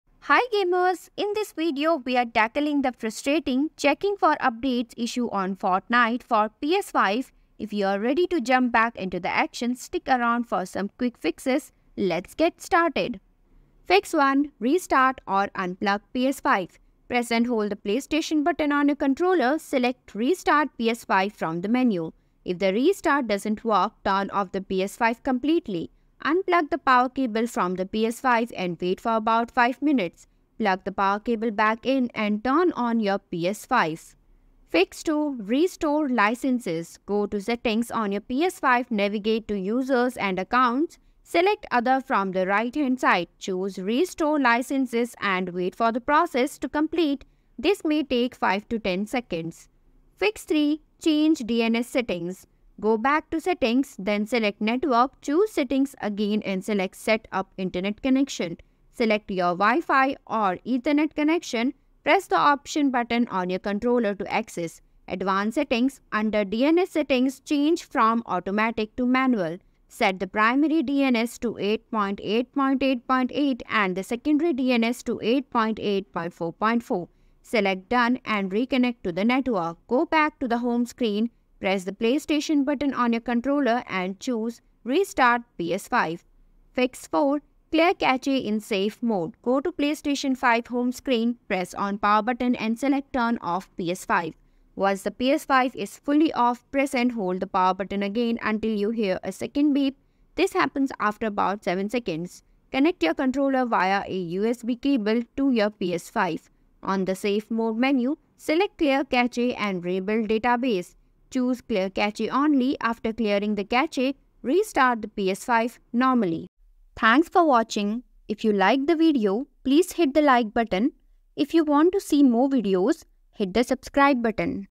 How To Fix Fortnite Stuck On Checking For Updates PS5
In this informative video, we provide a comprehensive guide on resolving the issue of Fortnite being stuck on the "Checking for Updates" screen on the PS5. Viewers will learn step-by-step troubleshooting techniques, including checking internet connectivity, clearing cache, and ensuring the game is updated. Join us as we help you get back into the action without further delay.

00:00 Introduction
00:13 Fix 1: Restart or Unplug PS5
00:34 Fix 2: Restore Licenses
00:49 Fix 3: Change DNS Settings
01:26 Fix 4: Clear Cache in Safe Mode